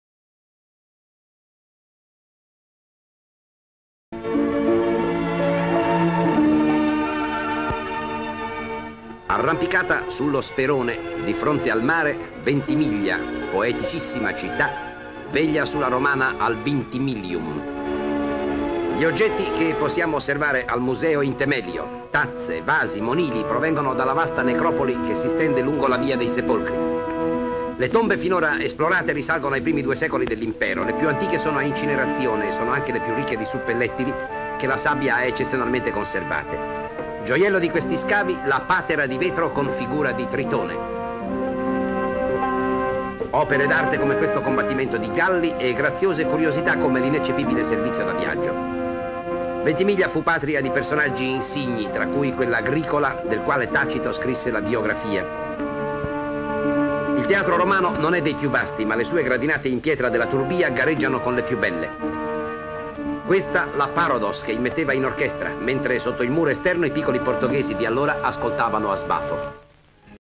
Ritrovamenti archeologici a Ventimiglia.
Descrizione sequenze:costa di Ventimiglia ; immagini di Ventimiglia ; reperti archeologici esposti al museo ; un uomo siede sulle gradinate del teatro antico ;